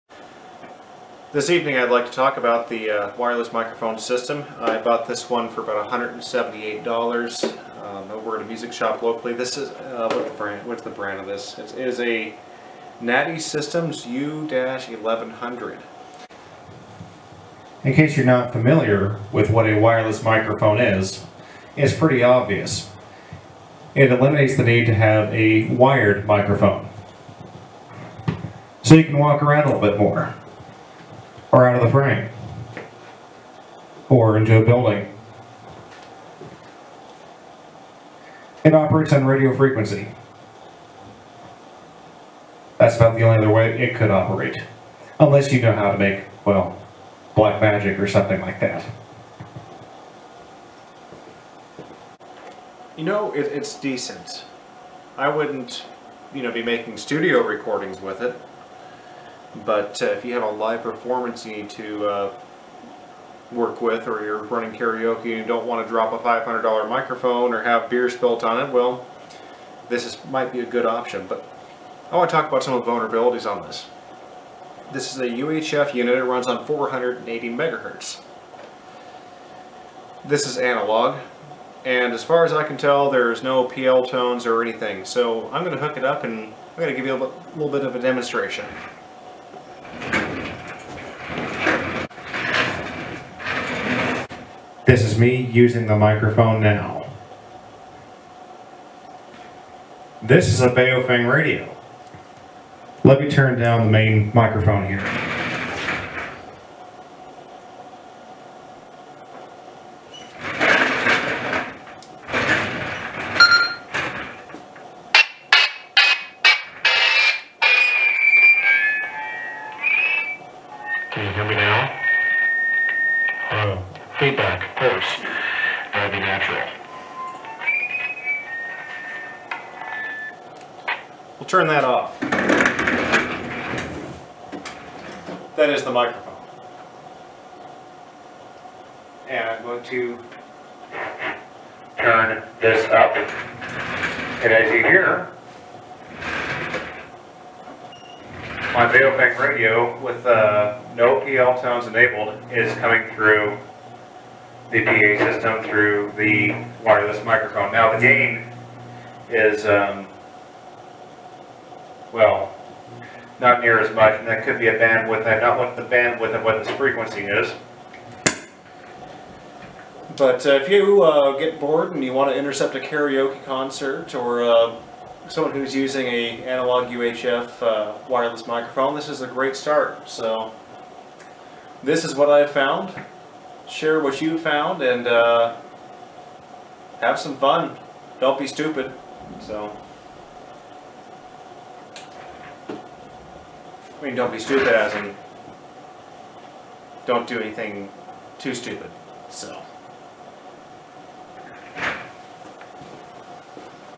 hacking wireless mics with a baofeng radio!
The equipment used here is a Nady Systems U-1100 UHF wireless mic kit, and a UV-5R Baofeng radio. The mic systems operates on a tunable frequency with the default set at 480 MHZ, which is within the range of the baofeng radio (a $25 radio you can get off of ebay). There does not appear to be any sort of PL tone, so this means your wide open to punch in the frequency and go. Side note, the baofeng being a 5 watt radio, you can easily dominate a wireless mic of this brand. One thing I observed is that the DB throughput from the baofeng was remarkably lower than that of the mic it self. Perhaps the bandwidth is wider on the mic it self... I really did not look in to it that deep... Disclaimer: This is for demonstration purposes only,  such transmissions can be illegal per FCC regulations. This was done in a basement in the middle of nowhere, where effecting anyone or anything is impossible.